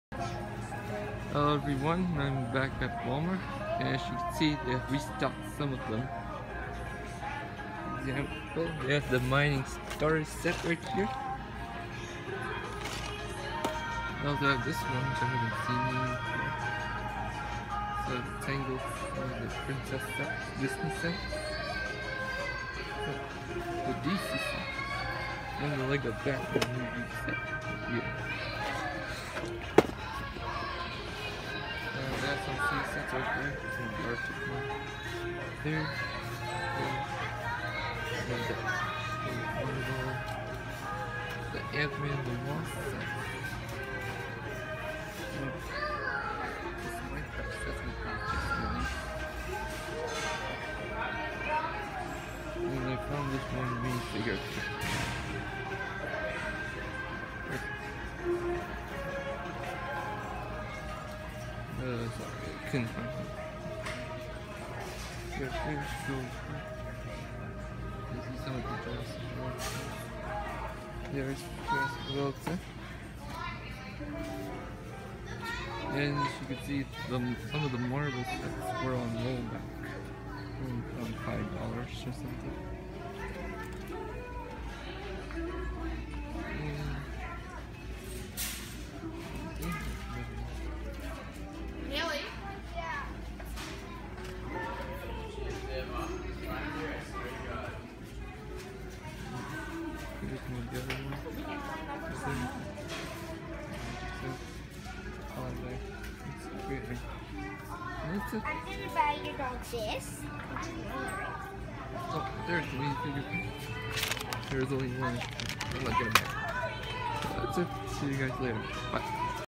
Lego Store Trip: Return to Walmart
Today, I went to Walmart (third time) and saw some Lego sets on shelves and rollback

The Sets that are new on shelves were:

-More Jurassic World
-76103 (Corvus Glaive Thresher Attack)
-DIsney Princess sets (Ariel's Wedding Ship)
-76109 (Quantum Realm Explorers)
-70919 (Justice League Anniversary Party)
-Some DC Sets
-21146 (The Skeleton Attack)
-Some City Arctic Sets